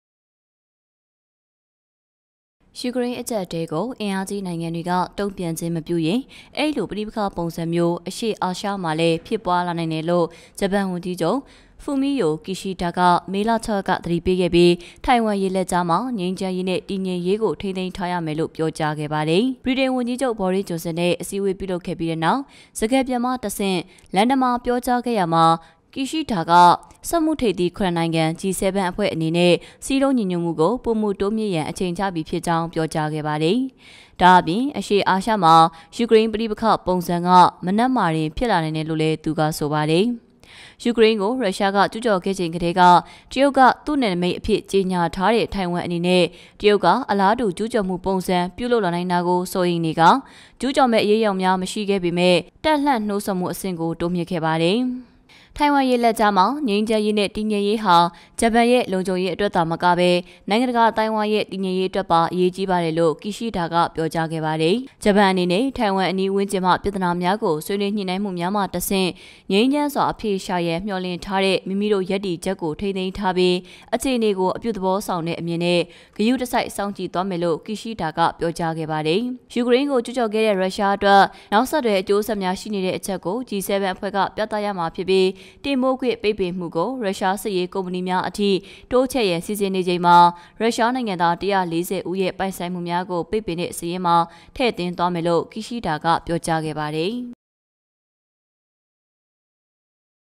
ယူကရိန်းအကျပ်အတည်းအပေါ် အင်အားကြီးနိုင်ငံများမှ မတုံ့ပြန်လျှင် ယင်းပဋိပက္ခပုံစံမျိုး
ယူကရိန်းအကျပ်အတည်းအပေါ် အင်အားကြီးနိုင်ငံများမှ မတုံ့ပြန်လျှင် ယင်းပဋိပက္ခပုံစံမျိုး အရှေ့အာရှတွင်လည်း ဖြစ်နိုင်ဟု ဂျပန်ဝန်ကြီးချုပ် သတိပေး

ယူကရိန်း အကျပ်အတည်းကို အင်အားကြီးနိုင်ငံတွေက တုံ့ပြန်ခြင်းမပြုရင် အဲဒီလိုပဋိပက္ခပုံစံမျိုး အရှေ့အာရှမှာလည်း ဖြစ်ပွားလာနိုင်တယ်လို့ ဂျပန်ဝန်ကြီးချုပ် ဖူမီ ယို ကီရှီဒါက မေ ၆ ရက်က သတိပေးခဲ့ပြီး ထိုင်ဝမ်ရေလက်ကြားမှာ ငြိမ်းချမ်းရေးနဲ့ တည်ငြိမ်ရေးကို ထိန်းသိမ်းထားရမယ်လို့ ပြောကြားခဲ့ပါတယ်။